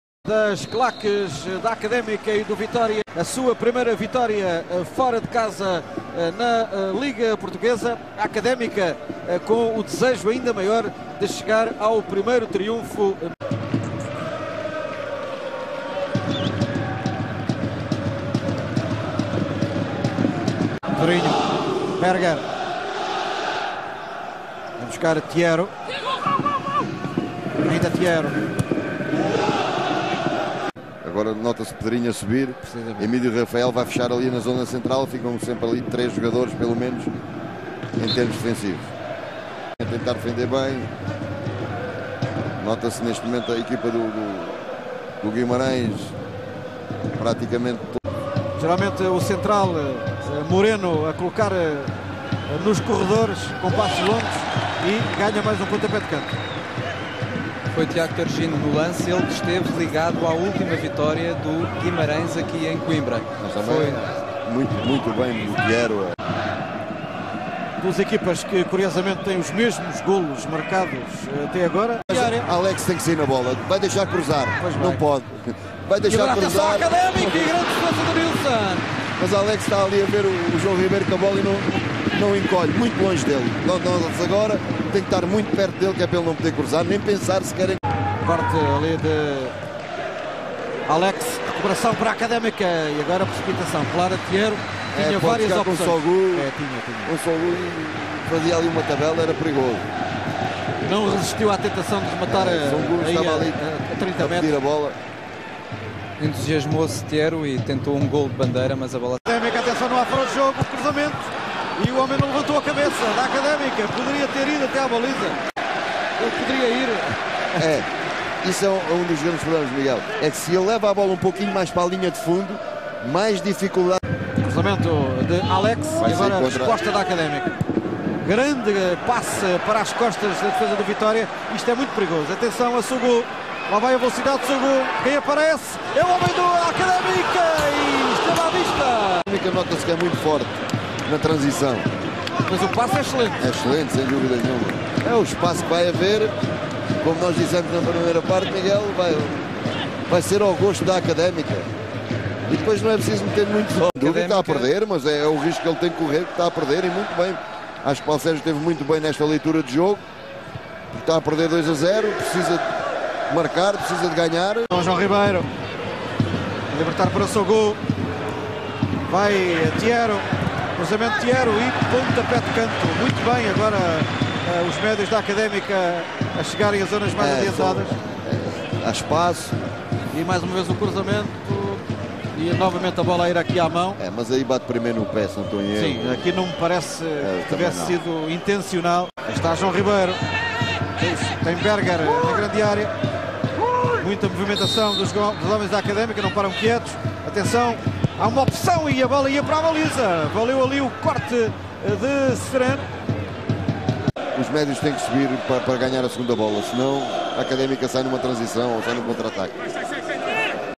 William Tiero - Liga Portugal, Academica vs Vitoria SC
William Tiero against his former club, Vitoria Guimaraes SC